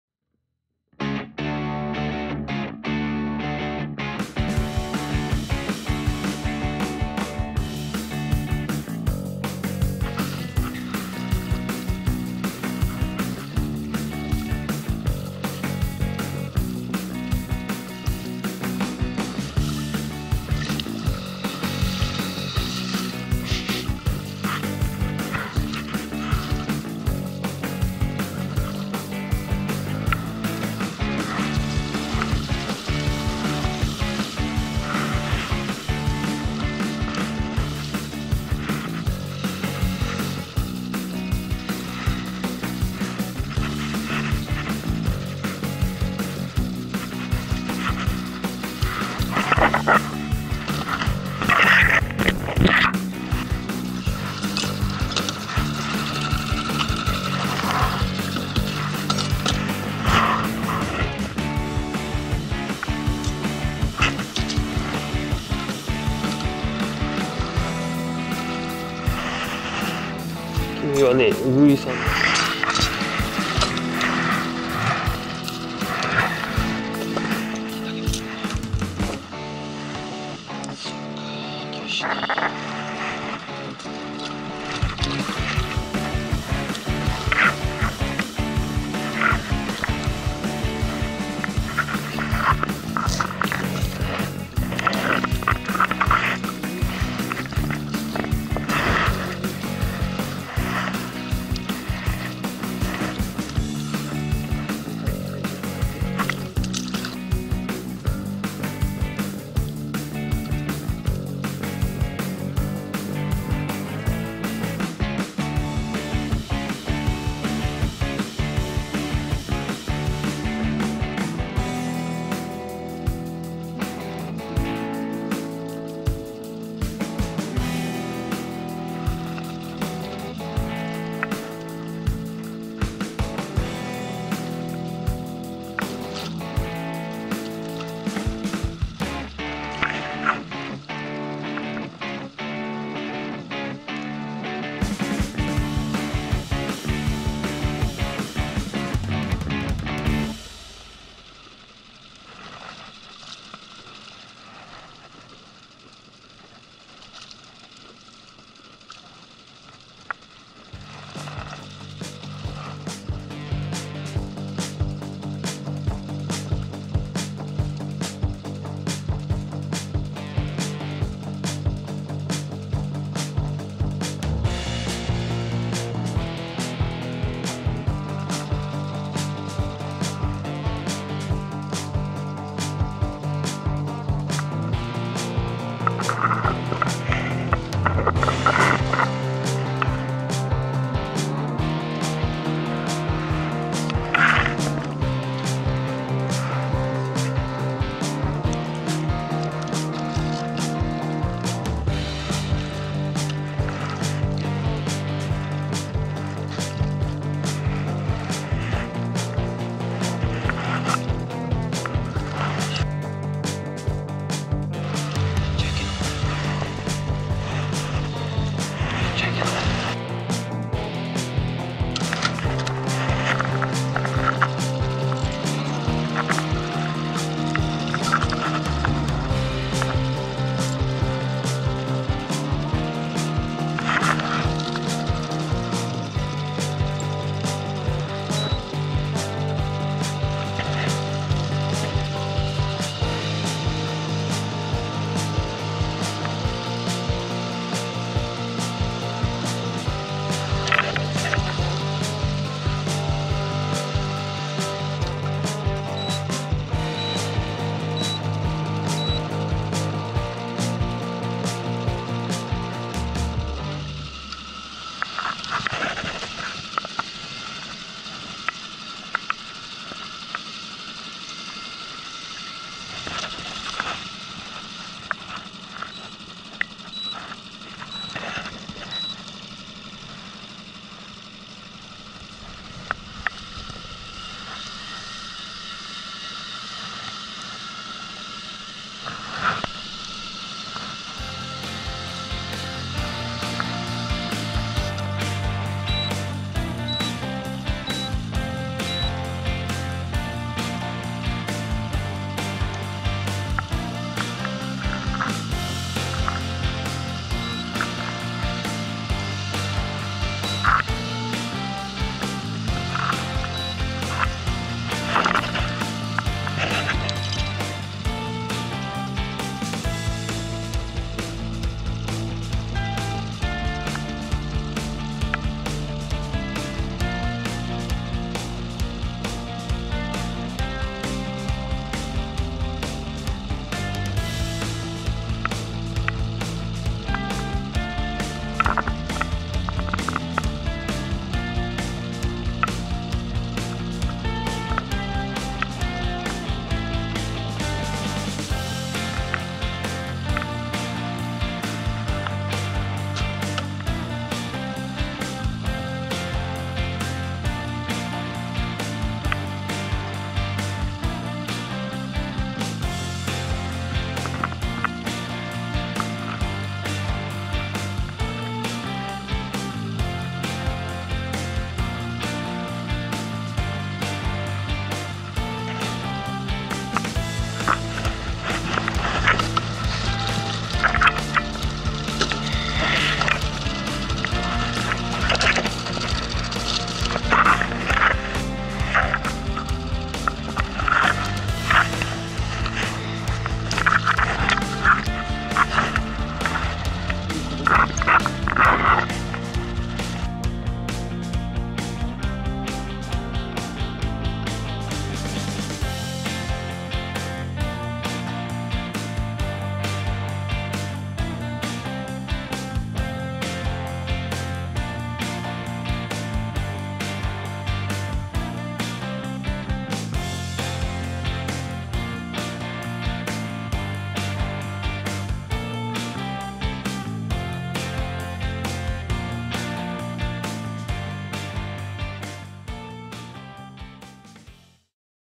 ビワマスシーズン第２戦！！前回からのUGUI地獄脱出なるか！？
今回も3週遅れの釣行動画。
前回に引き続き地元の大きな池でマス釣りです。

前回最後に深めに引いたのが吉と出たので、今日は最初っから深めで。
しかし、何も釣れません。前回釣れすぎて困ったUGUIも相手にしてくれません。お昼前に漸くあたったのは・・・・。